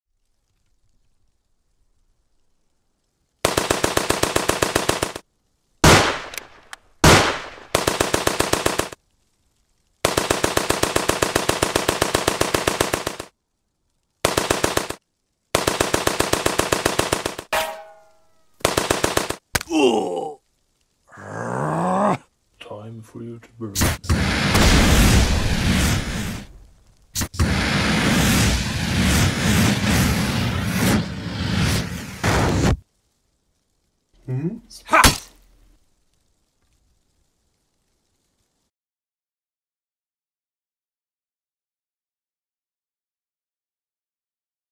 Lego ww1 stop motion short film
Since i am still waiting for parts so i can Continue  making my movie ( Goliath Attack ). I filmed this little ww1 short film over the weekend instead.